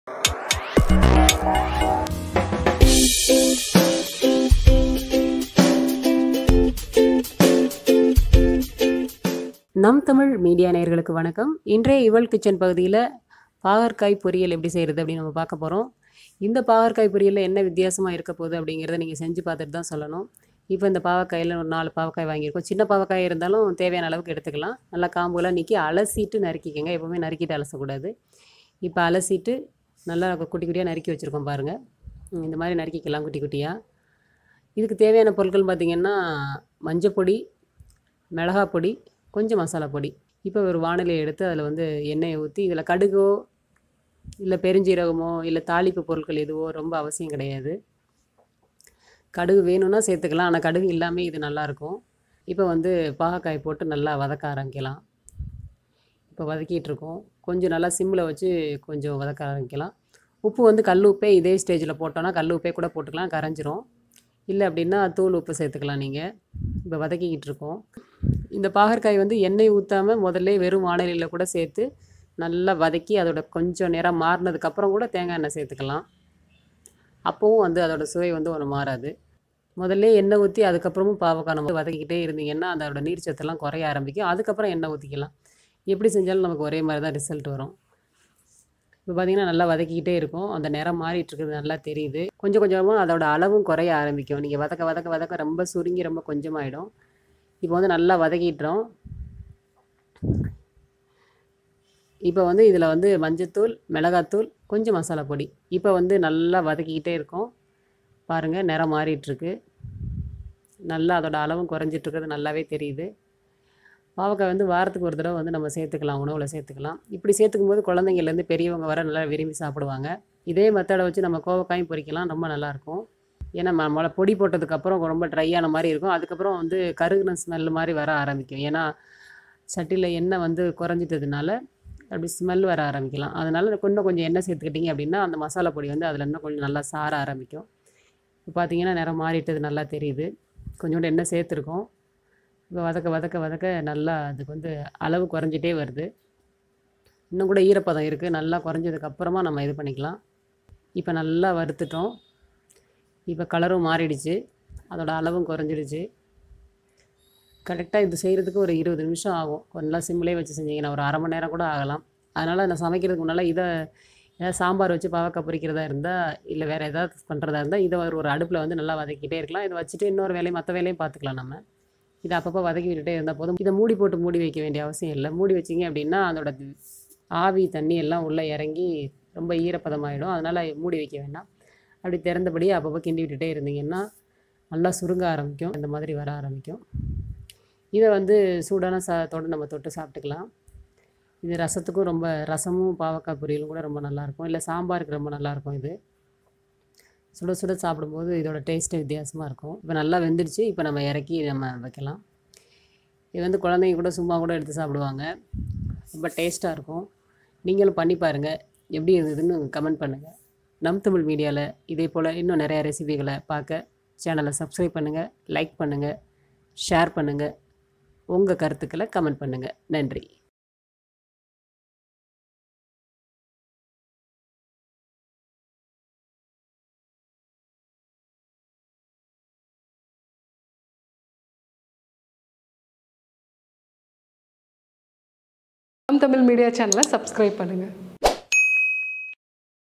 கசப்பு இல்லாத மொறு மொறு பாகற்காய் பொரியல் | இவள்கிச்சன் | நம் தமிழ் மீடியா
இவள் கிச்சன் பகுதியில் பாரம்பரிய மற்றும் புதிய ரெசிபிகளைக் காணலாம்

அடுப்பில்லாமல் 500க்கும் மேற்பட்ட ரெசிபிகளை செய்து அசத்தி வருபவர் படையல் சிவா. இங்கு சில ரெசிபிகளை நம் தமிழ் மீடியா நேயர்களுக்காக சொல்கிறார்.